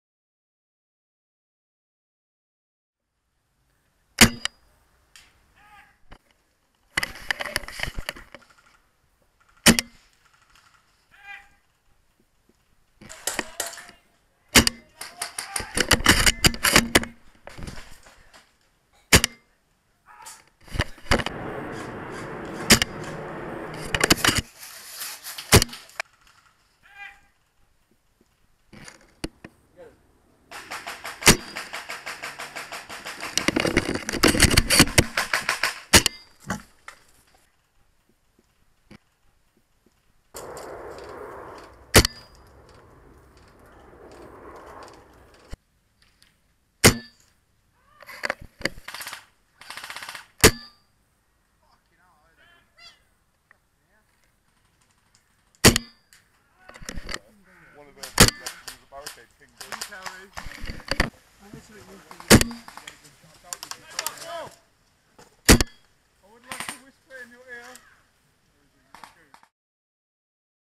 The Grange airsoft 2016 (zoom cam)
Just a quick few clips of some shots made while at The grange down in Balsall Common.

Friendly bunch of staff and a great site will be back soon